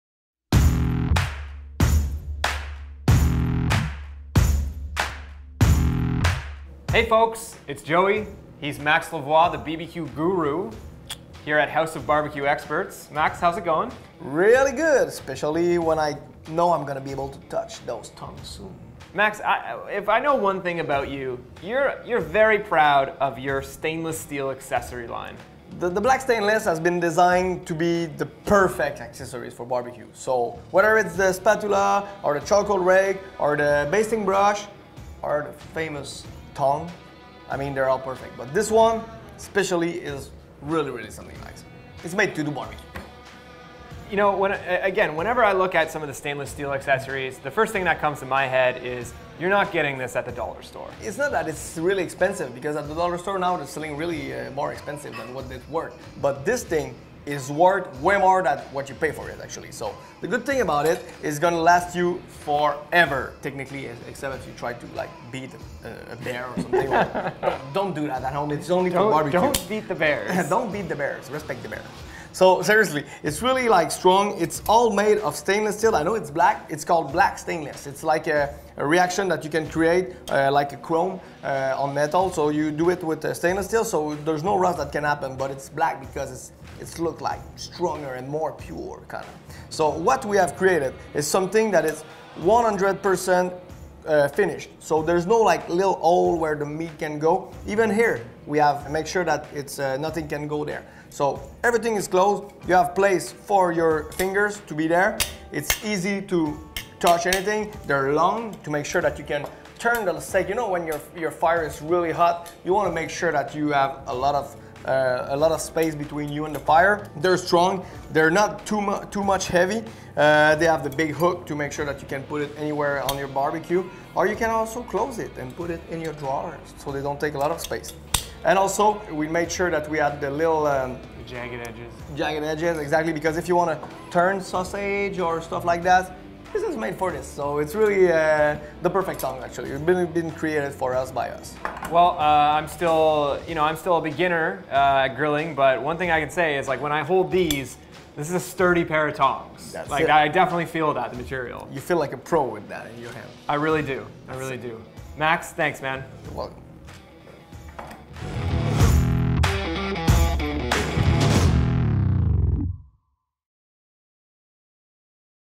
House of BBQ Experts - The Tongs
Joet and BBQ Guru Max Lavoie introduce you to the House of BBQ Experts black stainless steel Tongs that lets you grip grilled foods in style – while keeping a safe distance from heat sources.